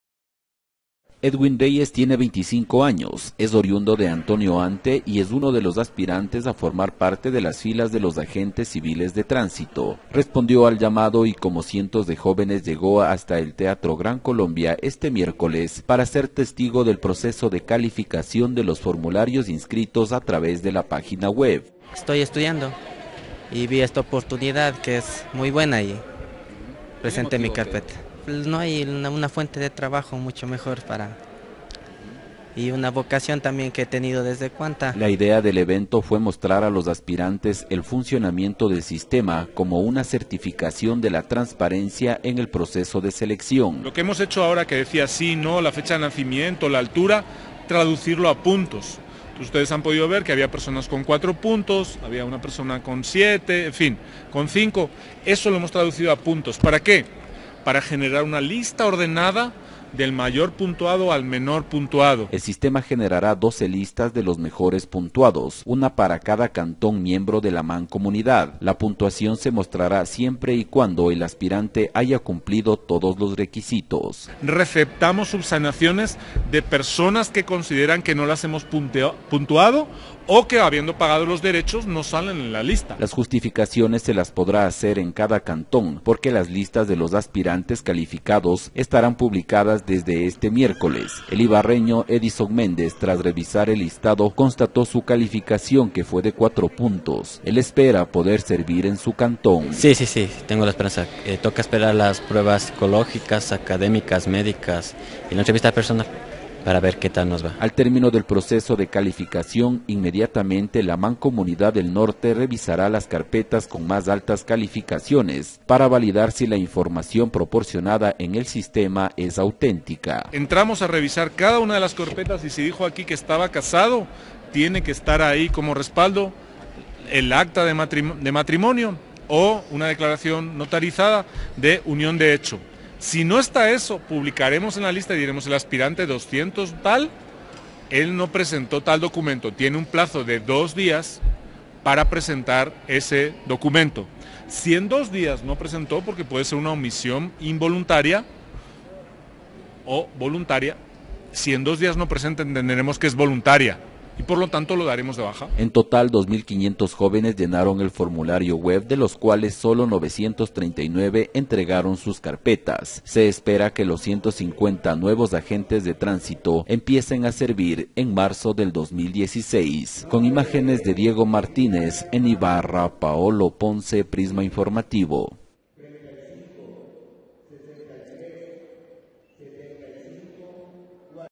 MOVIDELNOR, calificó a postulantes para Agentes Civiles de Tránsito (Noticias Ecuador)
Este miércoles, en el teatro Gran Colombia, la empresa pública de Movilidad del Norte, activó el sistema para calificar a los aspirantes a Agentes Civiles de Tránsito, registrados en la página web. Con este paso del cual fueron testigos por lo menos 1.000 personas, se generará la primera lista de interesados que continuarán en el proceso.